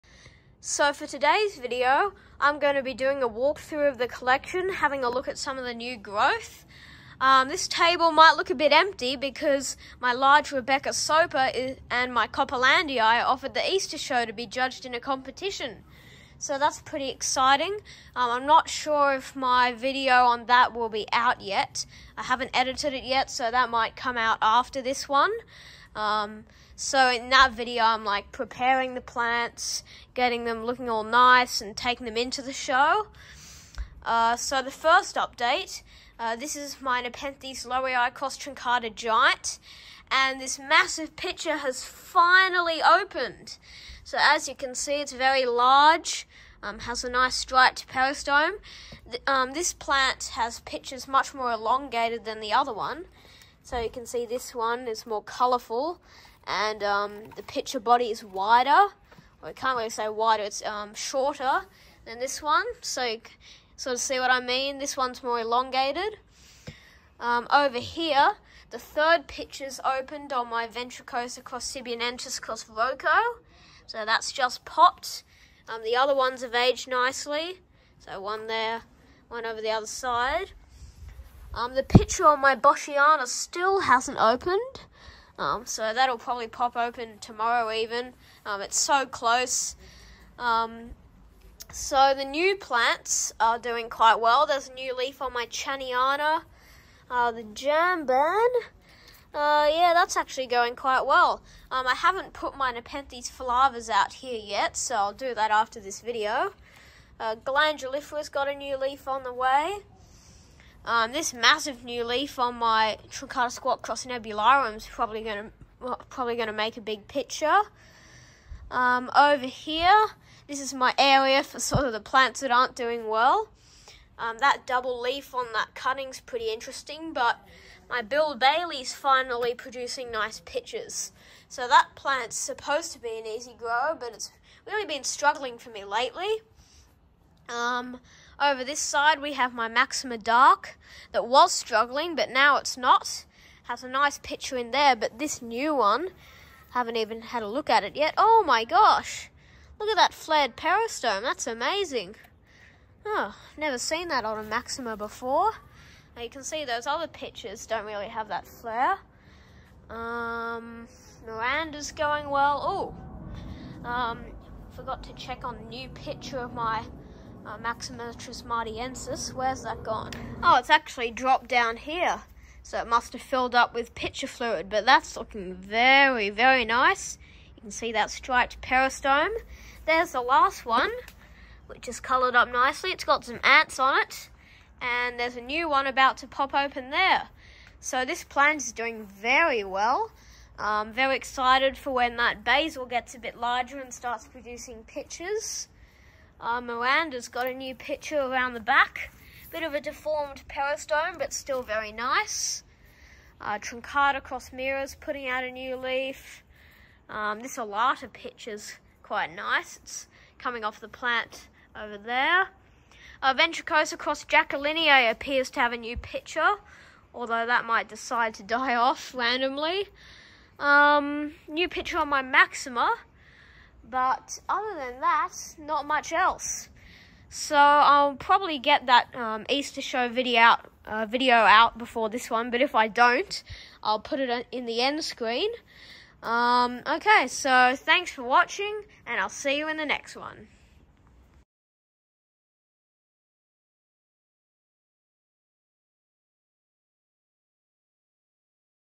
Nepenthes Updates - New pitchers and More!
In this video, I show some of the new growth on my plants!